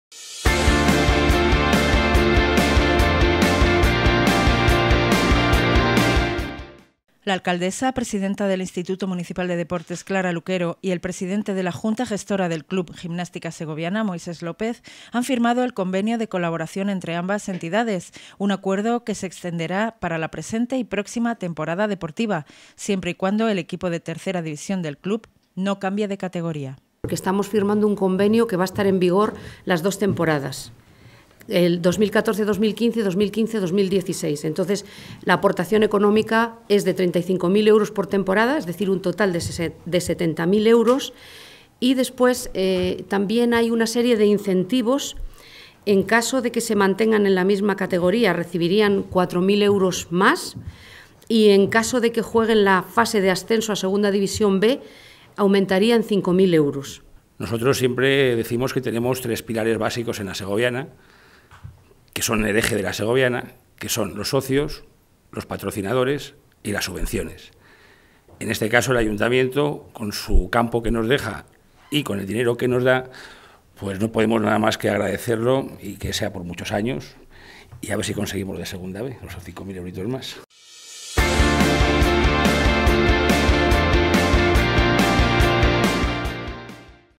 Convenio de colaboración Ayto. Segovia y Gimnástica Segoviana
La alcaldesa, presidenta del Instituto Municipal de Deportes (IMD), Clara Luquero, y el presidente de la Junta Gestora del Club Gimnástica Segoviana, Moisés López, han firmado el convenio de colaboración entre ambas entidades, un acuerdo que se extenderá para la presente y próxima temporada deportiva (2014/15, 2015/16) siempre y cuando el equipo de Tercera División del Club no cambie de categoría.